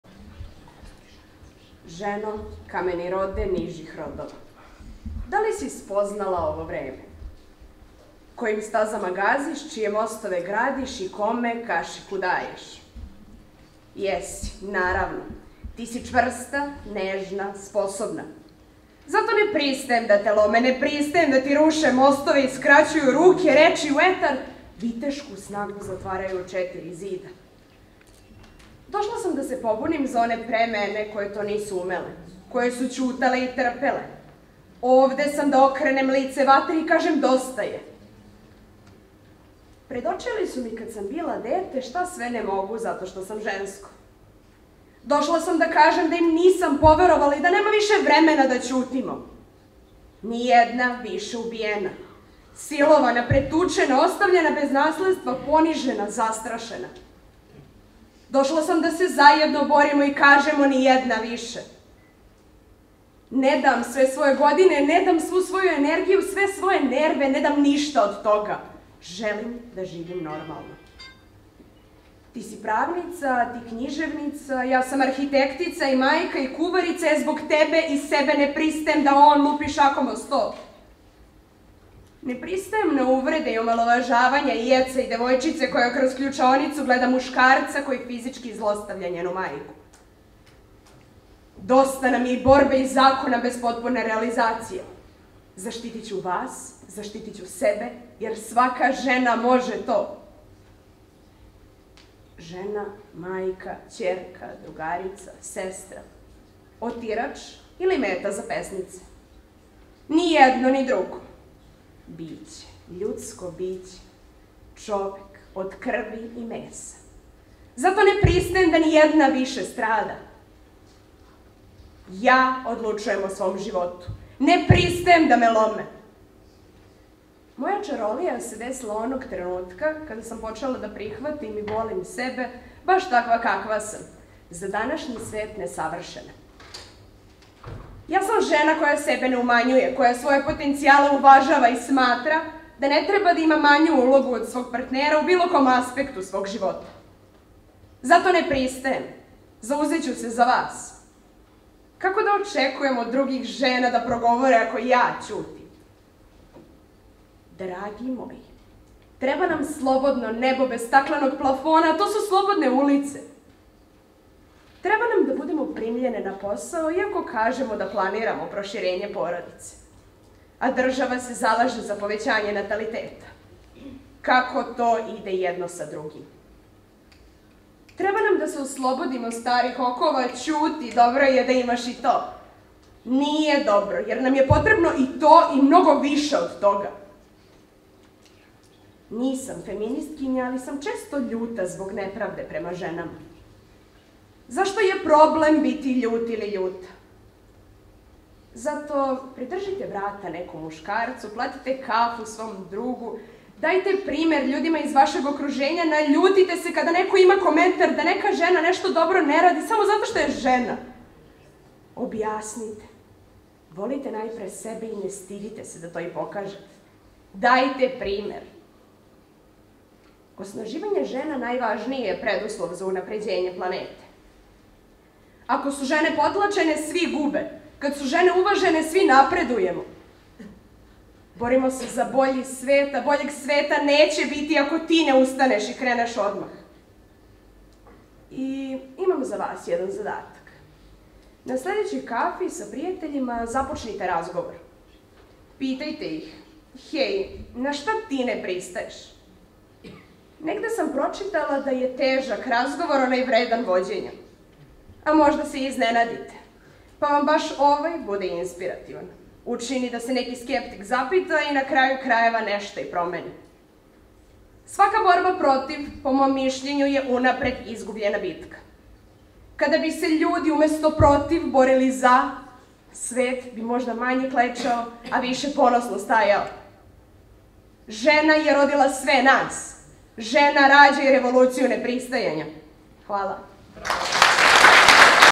Nađa Ristić, Gimnazija Jovan Skerlić Vladičin Han, drugoplasirana na takmičenju u besedništvu.
Svečana sala Gimnazije Bora Stanković, Vranje, 28. mart 2023. godine.